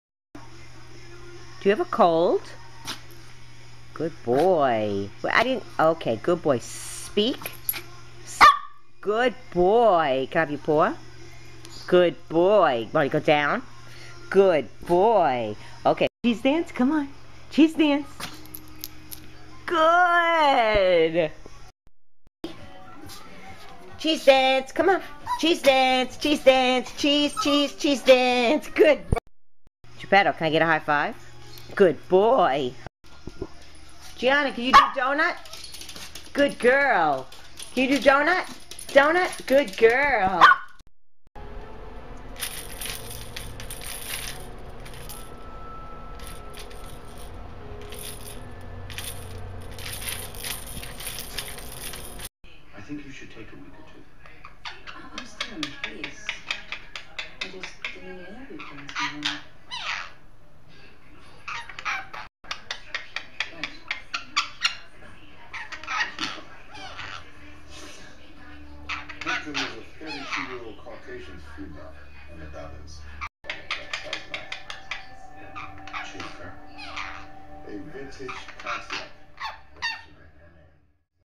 My Pets Do Tricks Montage 2011.MOV
Collcetion of tricks by - Giupetto and Gianna the dogs and DaVinci the talking bird says "How does a cat go? Meow. and How does a dog go? Woof Woof.